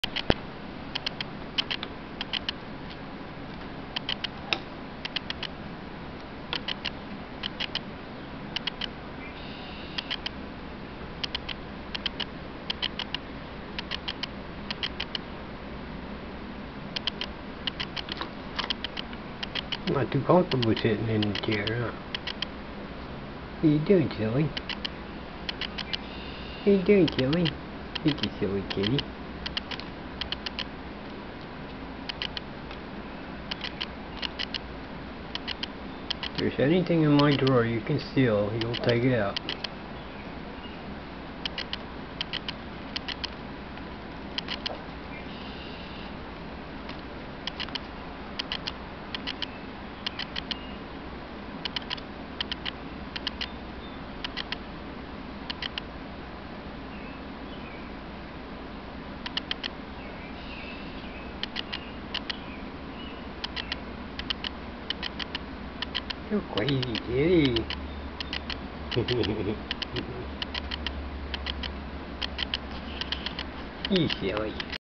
Coco stealing wire ties.mov
Cat trying to steal wire ties out of my Drawer. Video from my Kodak M340 DIGITAL CAMERA